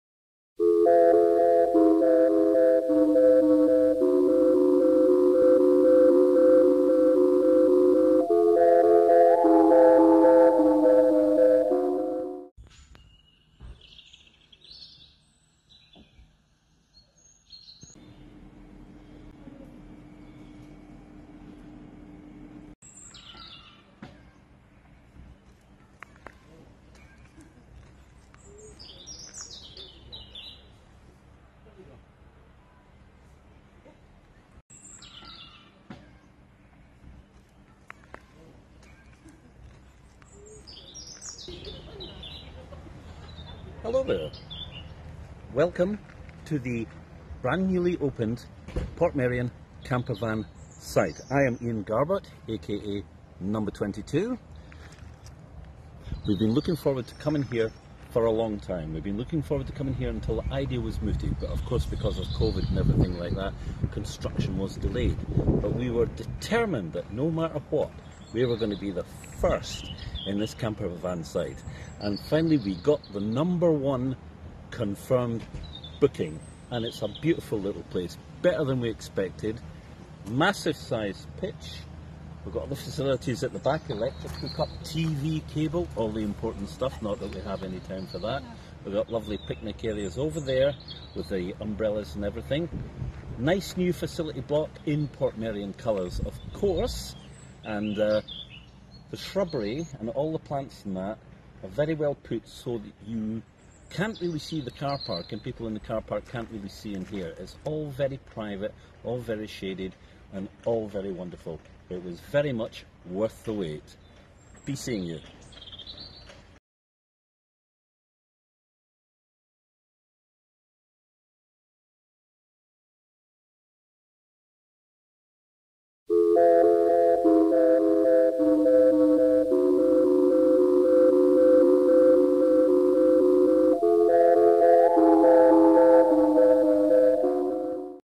SPEEDLEARN General MKII BROADCAST
The General MKII Villager supercomputer (as voted democratically by yourselves villagers) brings you SPEEDLEARN **Campervan Site Module**
A Village information film by Six of One Member Ian Garbutt (22)
Filmed and edited by Roy Stambrow